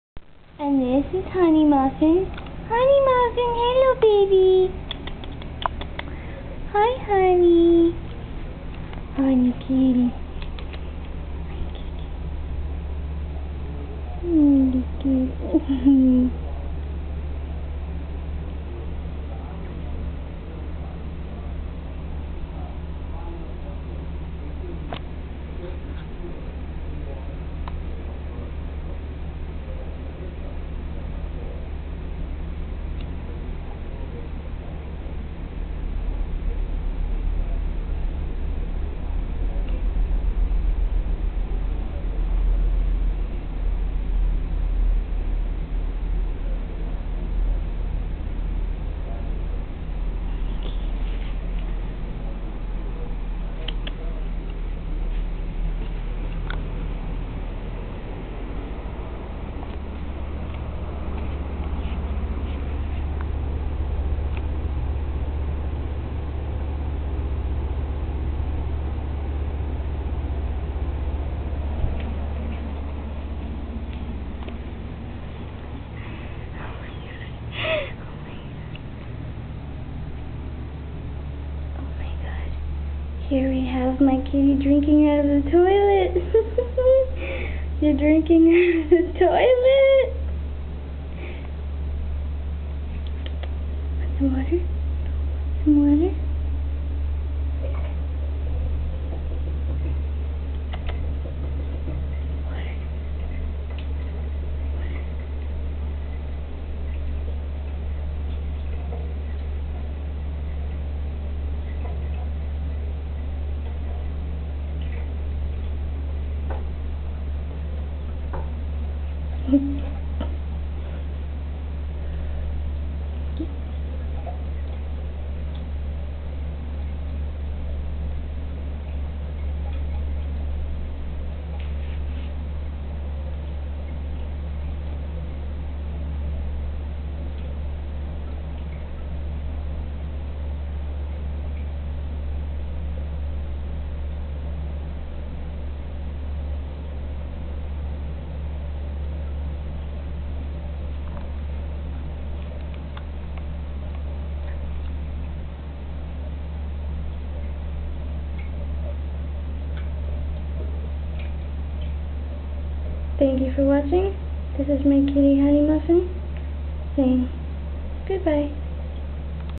what will my kitty do???
hi here's a vid of my kitty out and about around the house.. hope u like.. she's my baby!!!
also another test.. :)